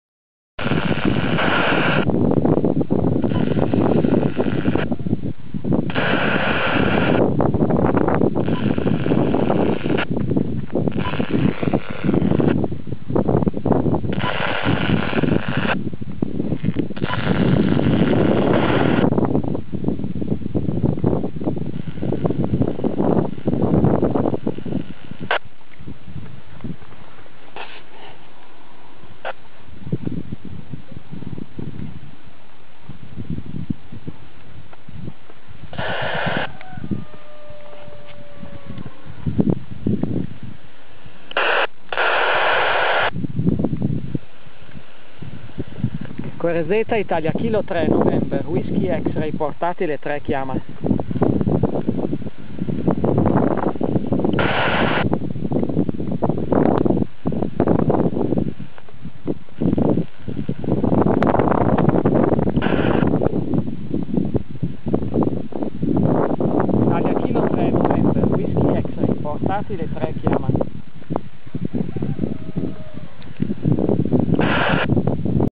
Testing Alinco DJ-G7 with 1,2Ghz PCB log antenna
Testing Alinco DJ-G7 with 1,2Ghz PCB log antenna. The PCB log antenna is homemade. Work very fine. 4-5 S point better than original rubber antenna.